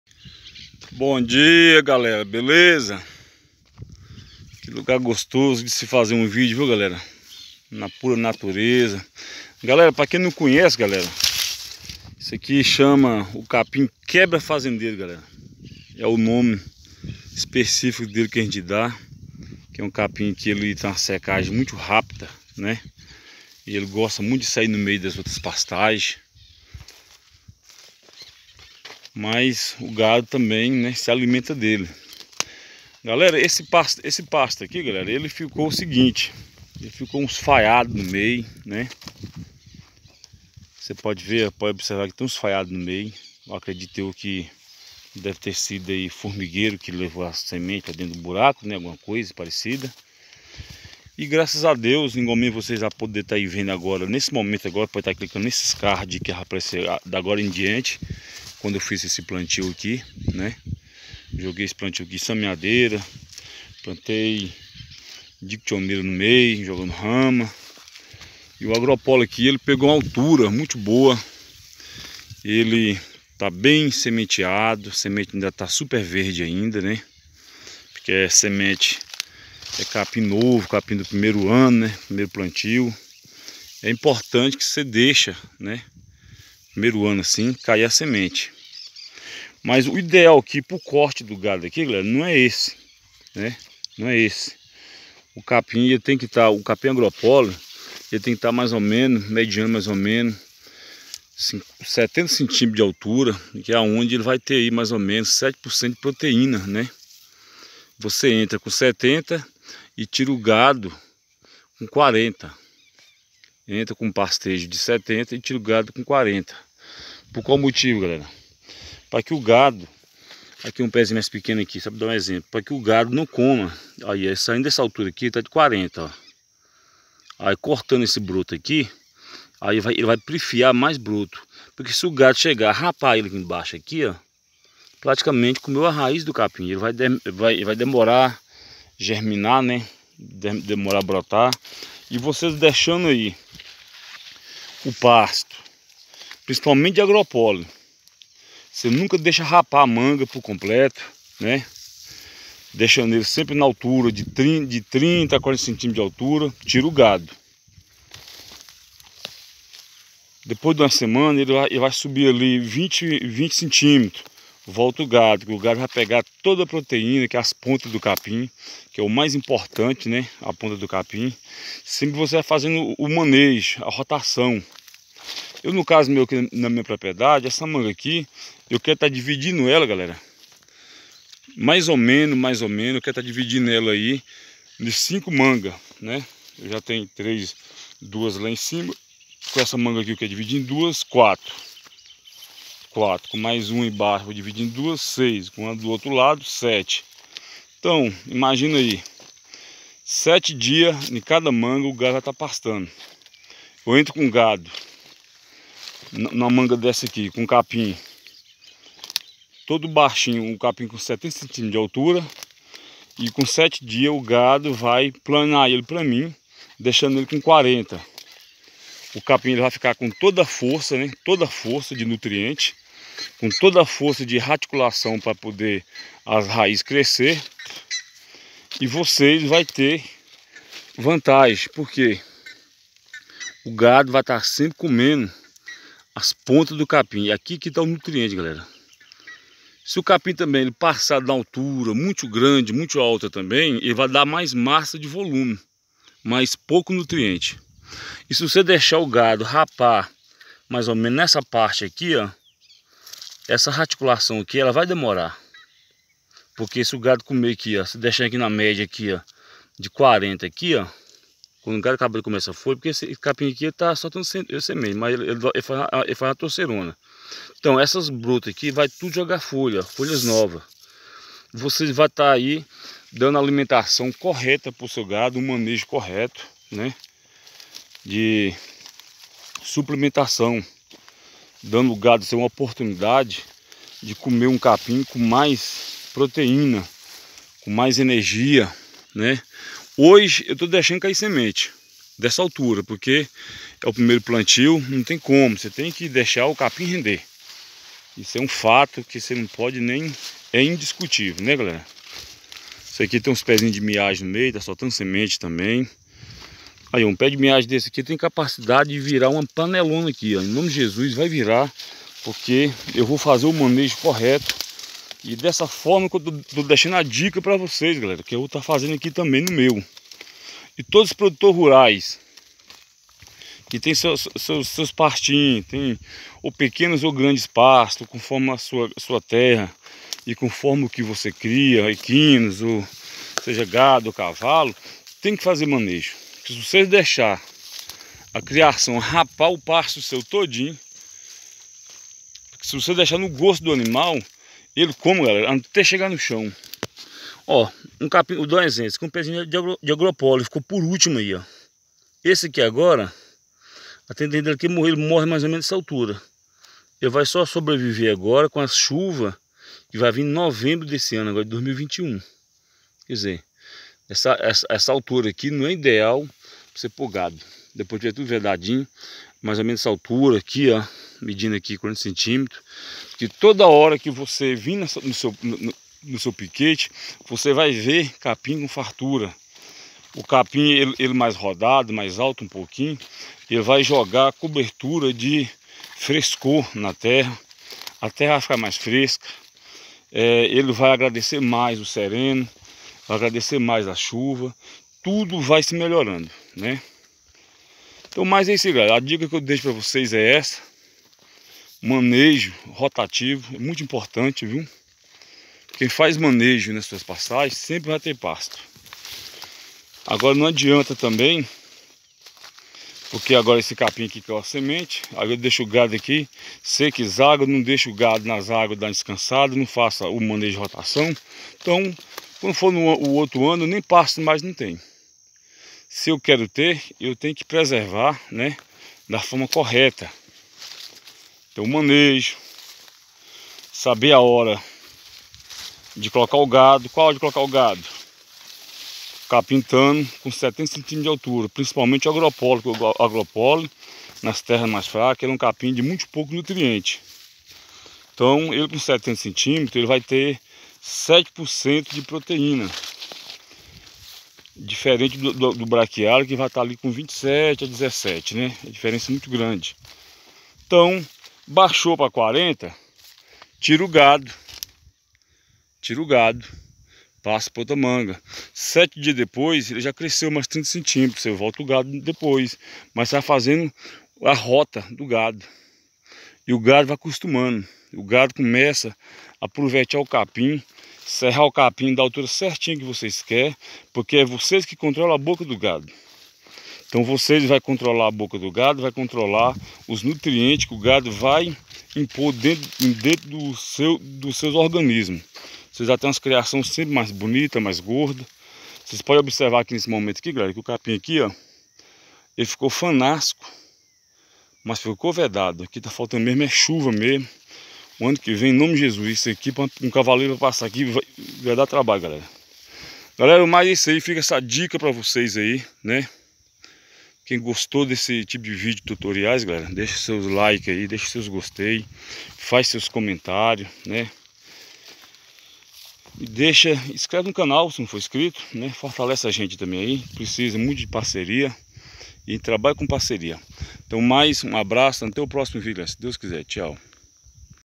MELHOR SALA DE AULA NO CAMPO É A EXPERIÊNCIA. QUAL É O PONTO IDEAL, PARA O CORTE E MANEJO DO CAPIM.
venha conhecer conhecimento adiquiridos com o tempo, de Autodidata: Josué Pereira Teixeira, falando do manejo do giro do gado nas pastagem/ MANEJO DO CORTE DO CAPIM AGROPOLE é MIYAGI.
   para que tenhamos mais ganhos de proteínas para os animais, é deixando os pastos sempre com com uma autura ideal, para mais rápido o seu crescimento. venha para essa aula pois pode ajudá hoje, é sempre continuamente.